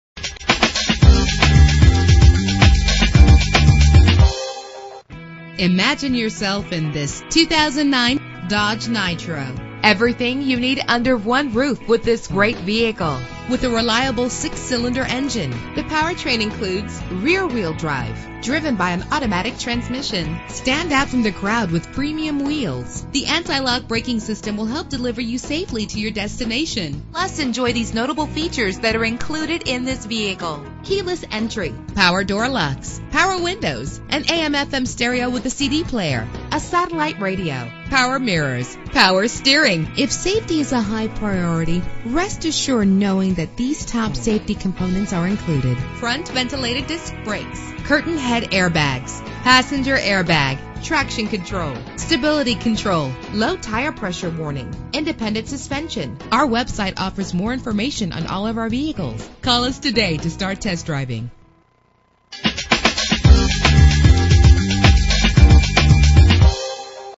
2009 Dodge Nitro - Tallahassee FL
Year: 2009
Make: Dodge
Model: Nitro
Trim: SE
Engine: 3.7 liter 6 cylinder 12 valve
Transmission: 4-Speed Automatic
Color: Inferno Red Crystal Pearlcoat
Mileage: 92155
Address:
3963 W. Tennessee Street